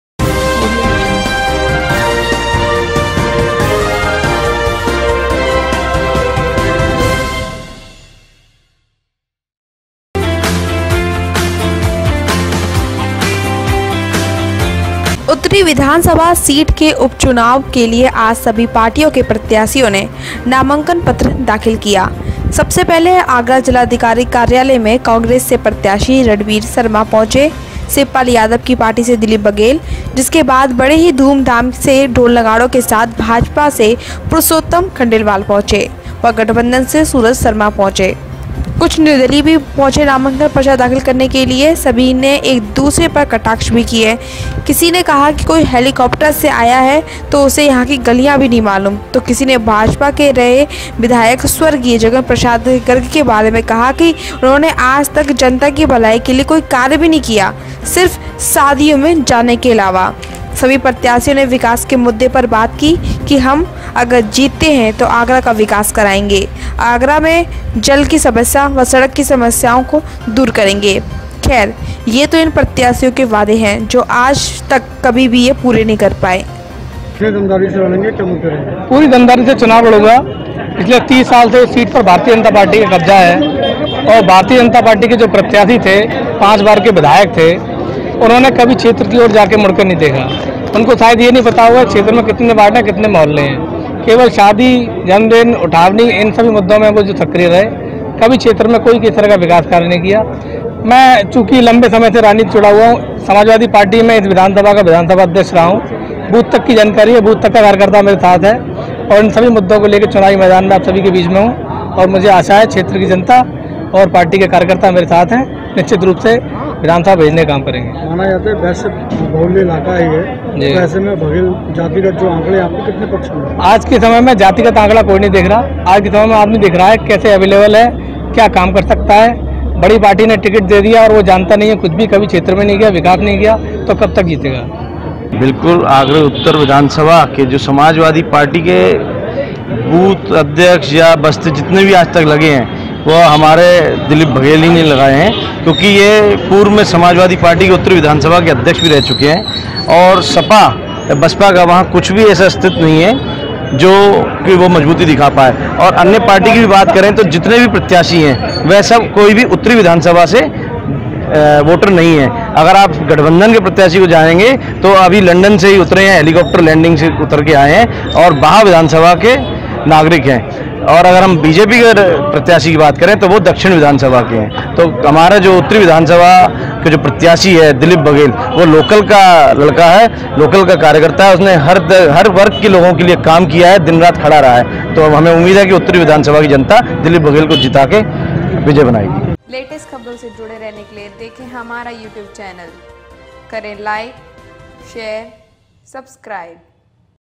अस्त्र न्यूज़ आगरा : नामांकन को बनाया प्रत्याशियों ने प्रतिष्ठा का प्रश्न, निकाले बढे बढे जलूस
उत्तरी विधानसभा सभा सीट के उप चुनाव के लिए आज सभी पार्टियों के प्रत्याशियों ने नामांकन पत्र दाखिल किया । सबसे पहले आगरा जिलाधिकारी कार्यालय में कांग्रेस से प्रत्याशी रणवीर शर्मा पहुंचे ,  शिवपाल यादव की पार्टी से दिलीप बघेल ,जिसके बाद  बड़े ही धूम धाम से ढोल नगाड़ों के साथ भाजपा से पुरूषोतम खंडेलवाल पहुंचे , व गठबन्धन से सूरज शर्मा पहुंचे । कुछ निर्दलीय भी पहुंचे नामांकन पर्चा दाखिल करने । सभी ने एक दूसरे पर कटाक्ष भी किए । किसी ने कहा कि कोई हेलीकॉप्टर से आया है उसे तो यहां की गलियां भी नहीं मालूम तो किसी ने भाजपा के से रहे विधायक स्वर्गीय जगन प्रसाद गर्ग के बारे में कहा कि उन्होंने आज तक जनता की भलाई के लिए कार्य नहीं किया सिर्फ शादियों में जाने के इलावा।सभी प्रत्याशियों ने विकास के मुद्दे पर ही बात की । कि हम अगर जीते तो आगरा का विकास कराएंगे । आगरा में जल की समस्या ,सड़क की समस्या को दूर करेंगे । खेर ये  तो इन प्रत्याशियों के वादे है जो आज तक कभी भी ये पूरे नहीं करपाये